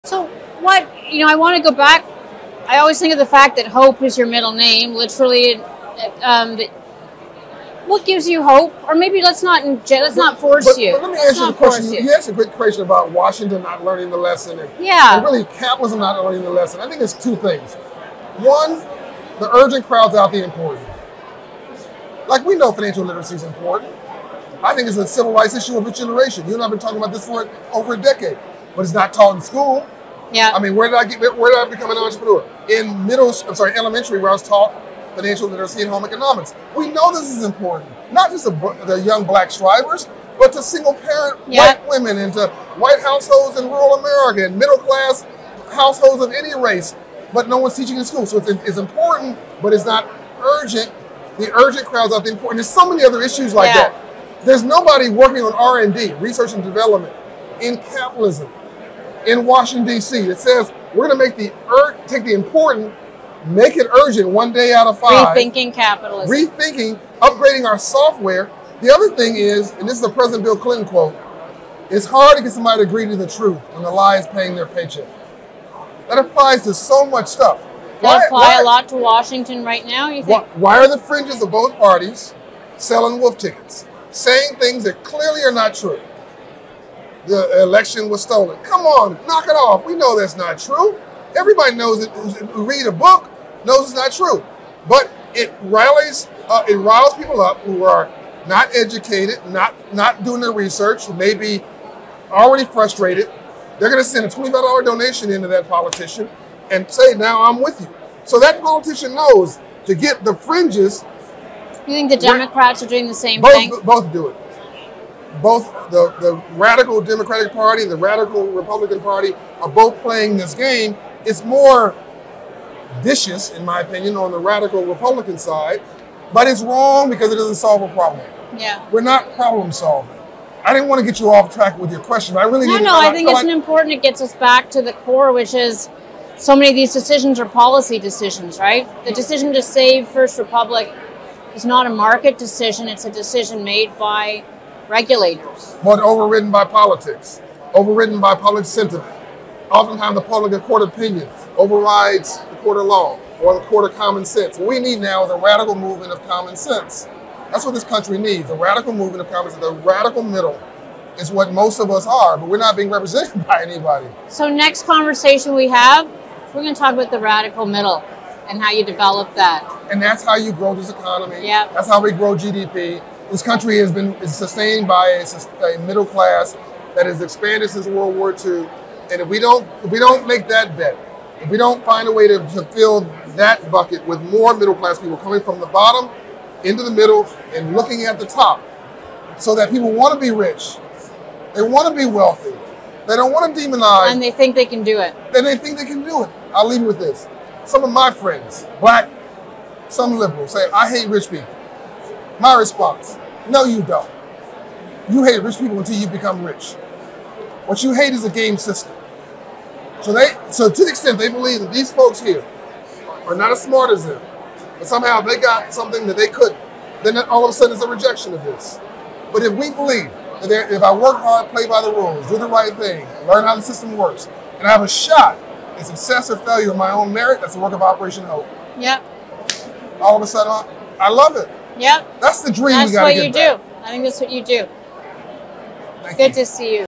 'What You Hate Is A Gamed System': John Hope Bryant Breaks Down Middle Class Stagnation And Apathy
John Hope Bryant, Founder, Chairman, and CEO of Operation HOPE, joins "Forbes Newsroom" to discuss what gives him hope and how the political polarization drives